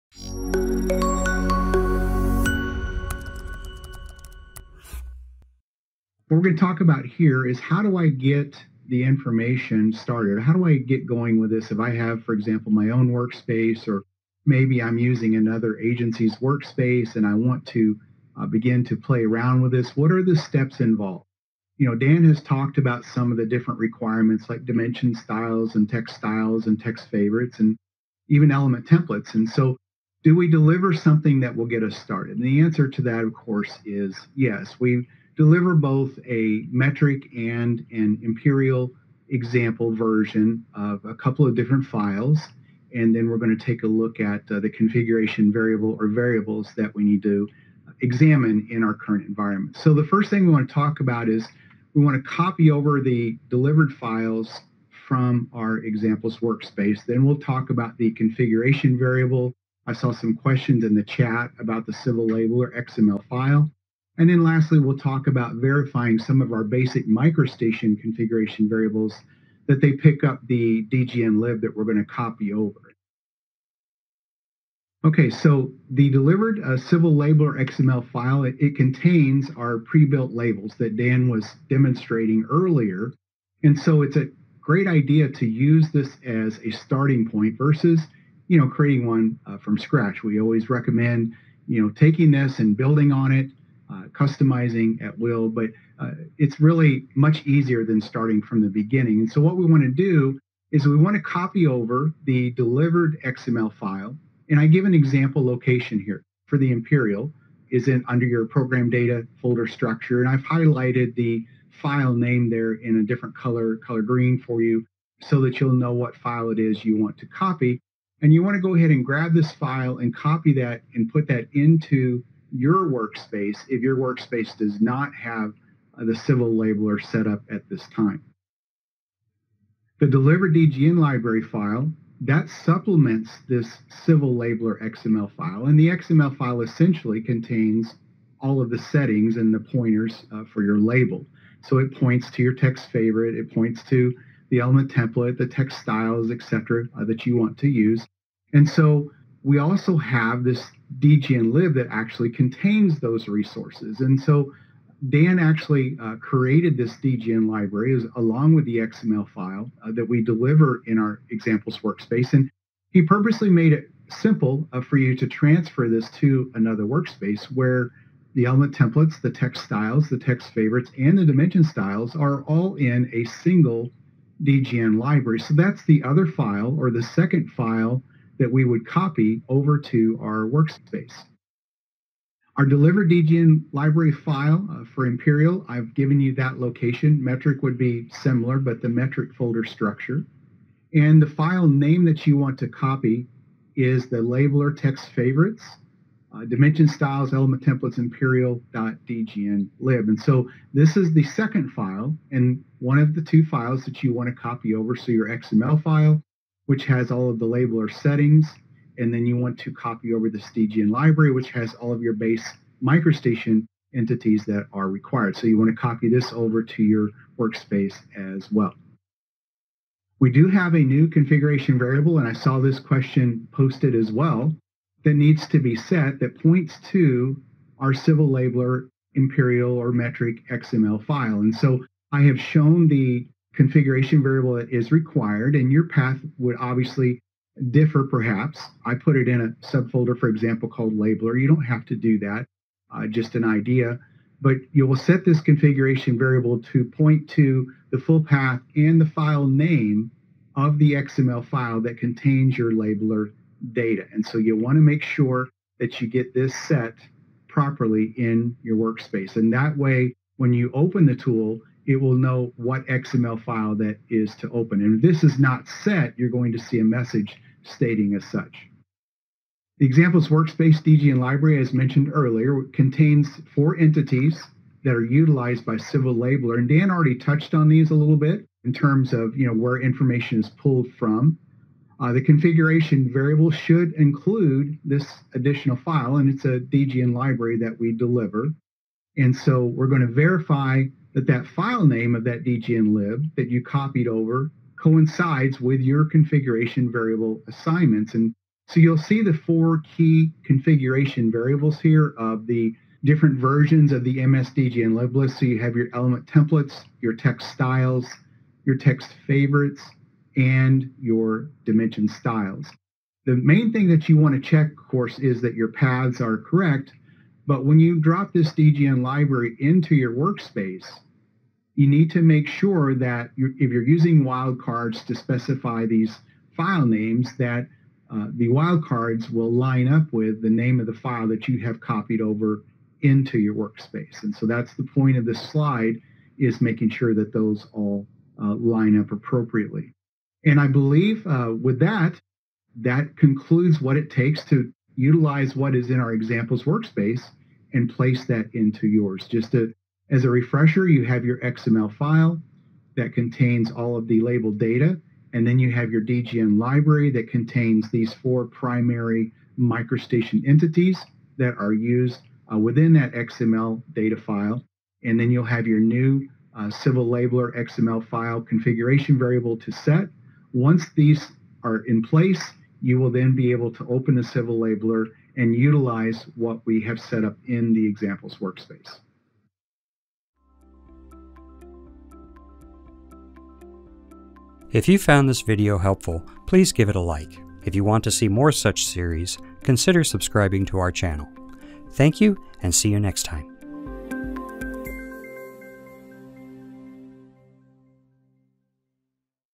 Civil Labeler Part 05: WorkSpace Implementation
In this video, you will learn how to take the delivered civil labeler .xml file and integrate it with your workspace.